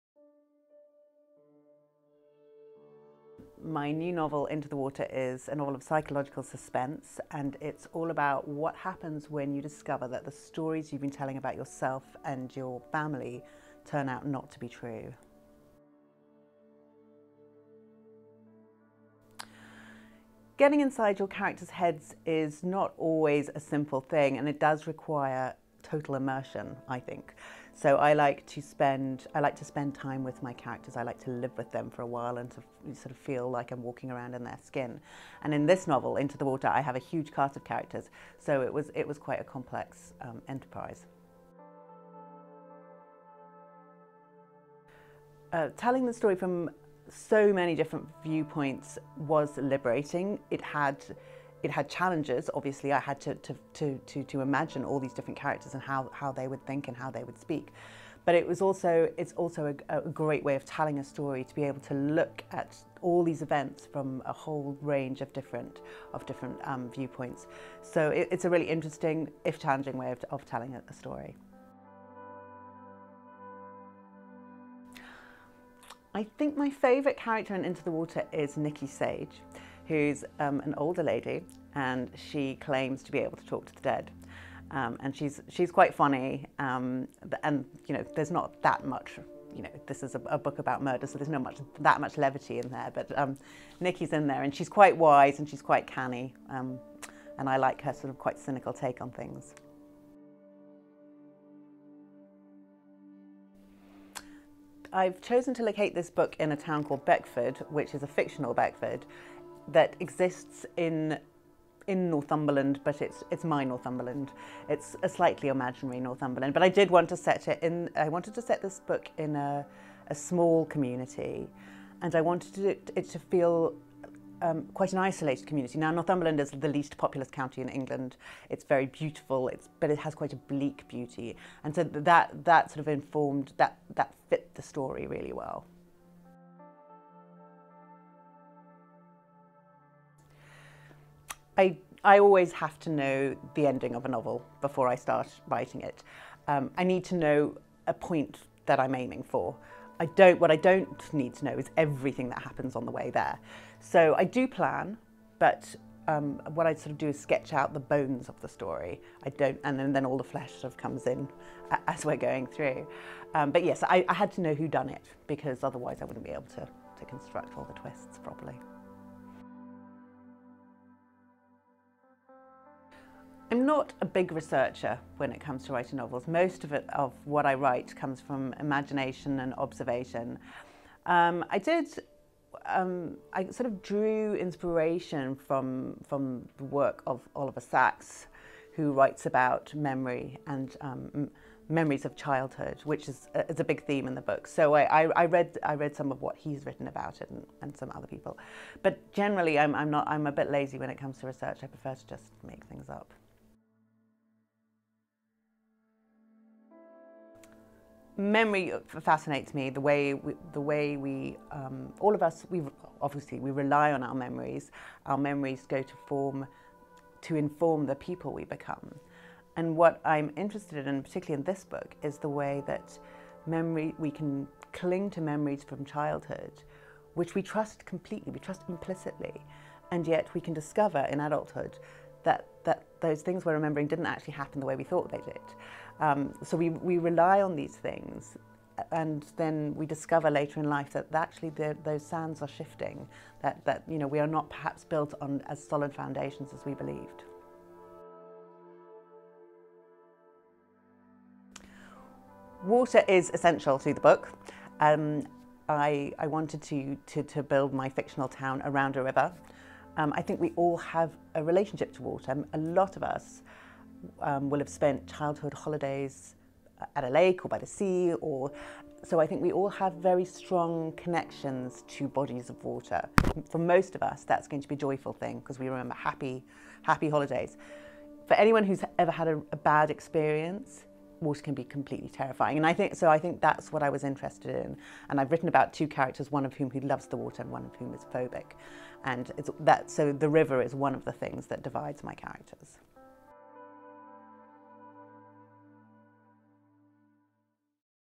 Exclusive Interview! Paula Hawkins on Into The Water
Set in a small Northumberland town where a number of women have mysteriously died in the river over the years, Into the Water introduces us to Jules who returns to the town after her estranged sister Nel is found drowned. Nel was obsessed with the river, whereas Jules is still afraid of the water. As she confronts Nel’s teenage daughter, the police and a town full of characters who are hiding secrets, Jules must not only face up to the reasons why her sister ended up dead in the water, but the memories she has of her hometown that drove her from her away in the first place.

We caught up with Paula ahead of her second big release to find out more about her latest psychological thriller.